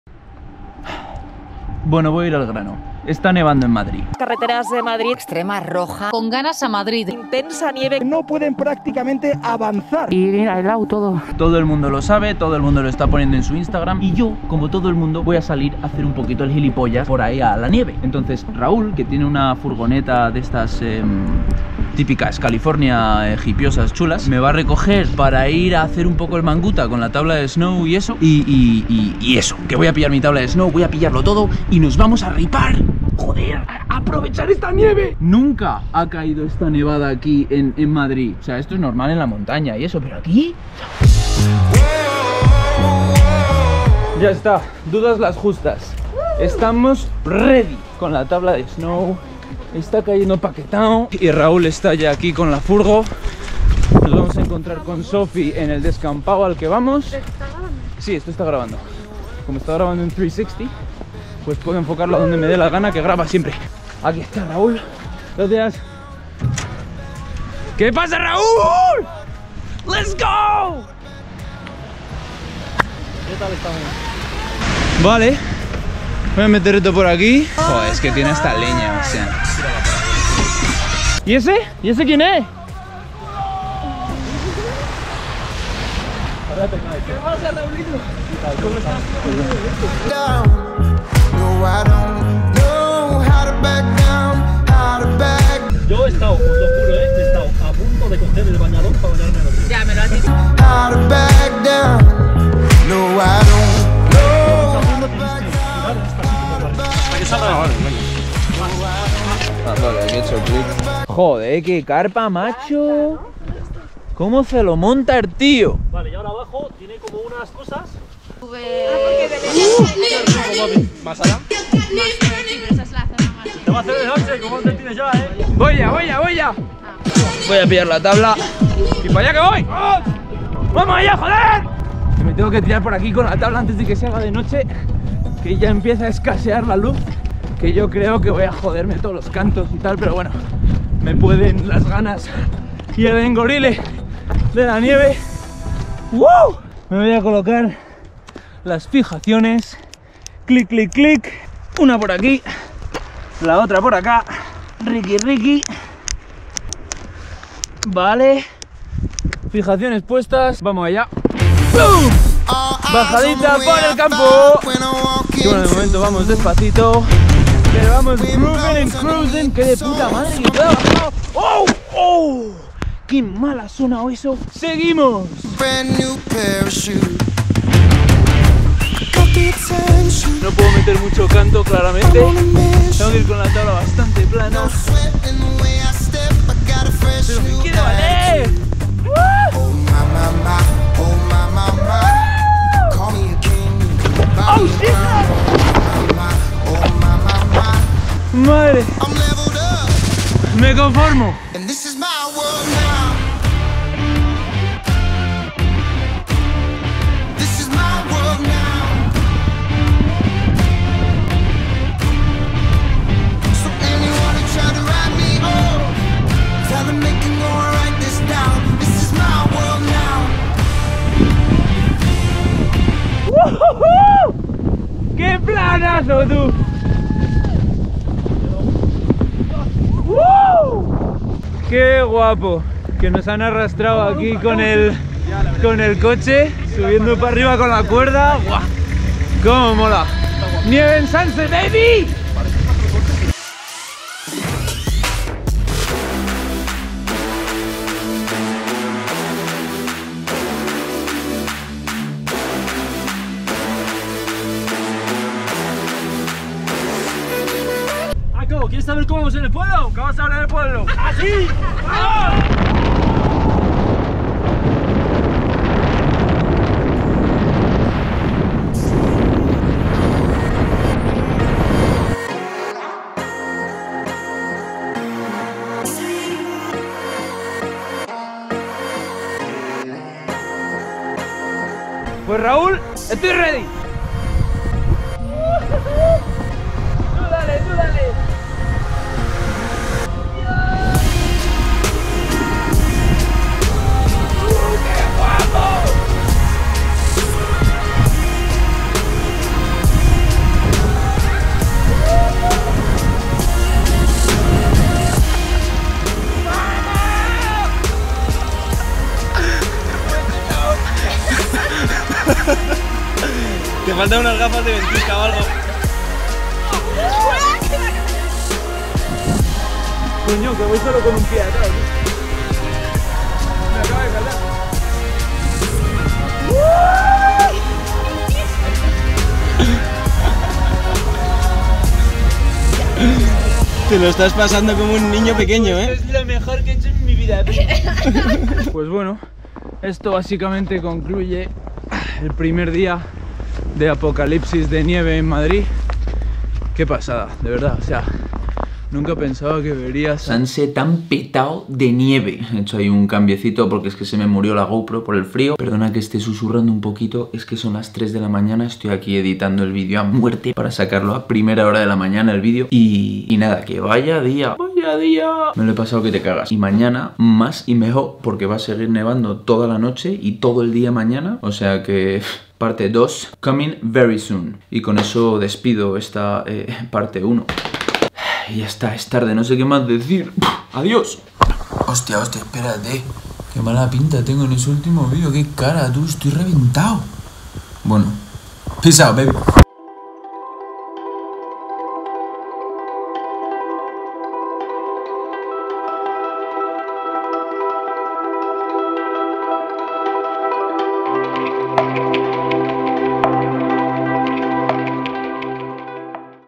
❄️ MADRID COLAPSADO por NIEVE: SNOWBOARDING por las CALLES!
Con la nevada extrema que ha caído en Madrid, no podía dejar pasar la oportunidad para salir con la tabla de snow y aprovechar como un enano!!
Video cortito pero jugosito, y en breves subo otro! Que ha seguido nevando y en el momento que estoy subiendo esto, mi terraza tiene 50cm de nieve ya.

Gracias Raul por montar planazos brutales siempre entre tu furgo, la carpa y la hoguera - ENORME.

Déjame un comentario si te ha gustado el video!!!
GRACIAS!!